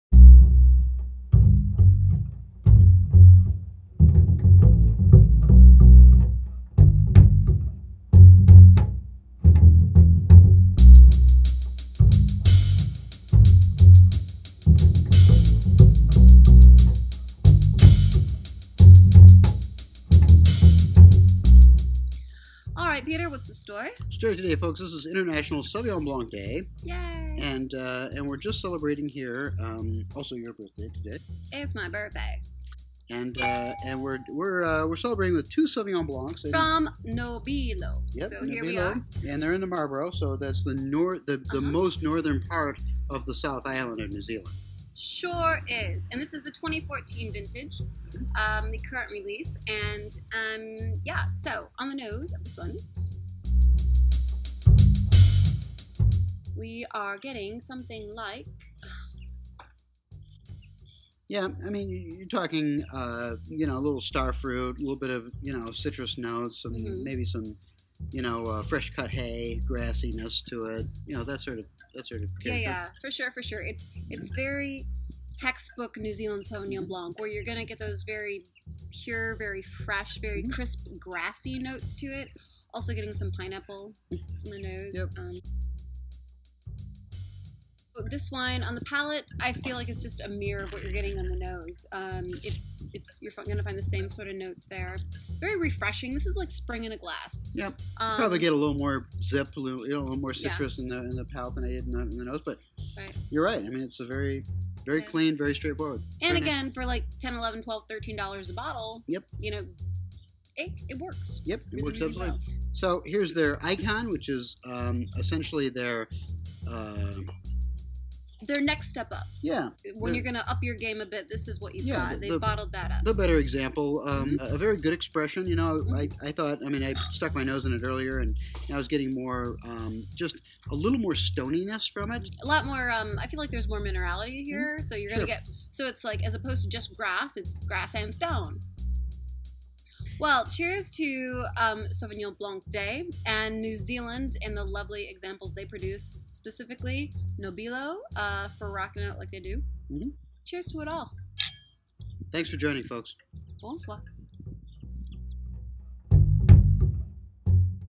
Nobilo 2014 Sauvignon Blanc
To celebrate Sauvignon Blanc day this year, Peter and Allie select Nobilo to sip. Two different versions - the entry level and the 'Icon'. Both refreshing, with one a bit more refined, for a duo of grassy, herbaceous goodness both on the nose and on the palate. Cheers to New Zealand Sauvignon Blanc & Nobilo!